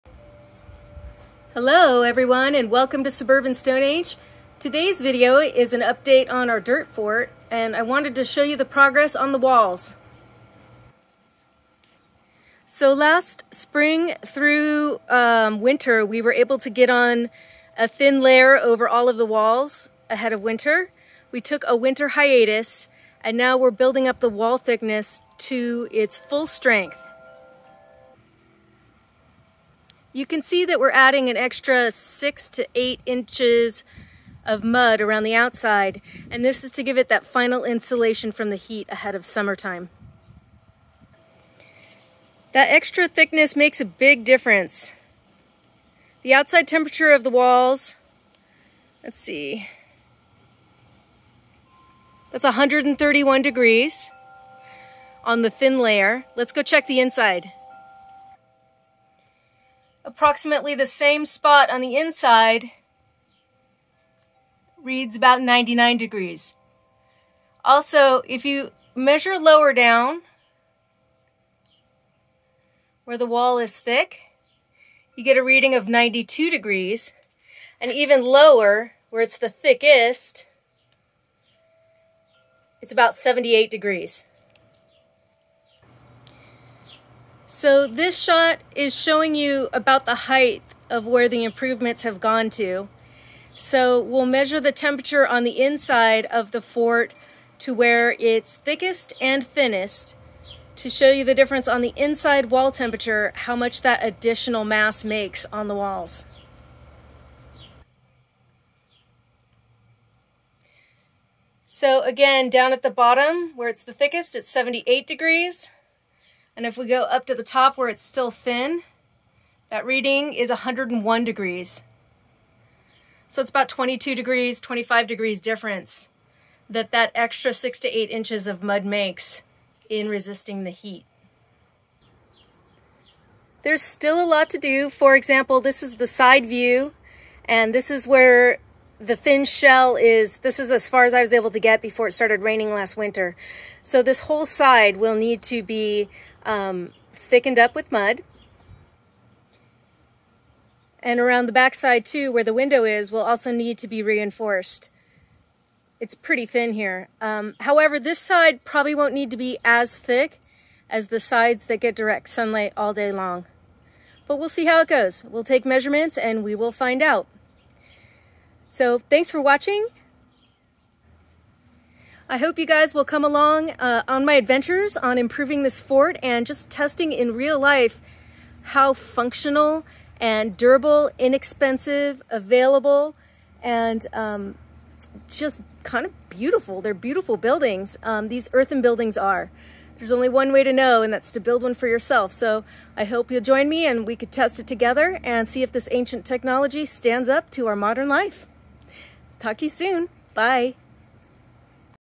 Thick Earthen Walls Beat the Heat - Primitive Survival Fort
In a race against the summer heat, I am adding on another layer of earth to the dirt fort walls.  I take measurements to show you the difference it makes!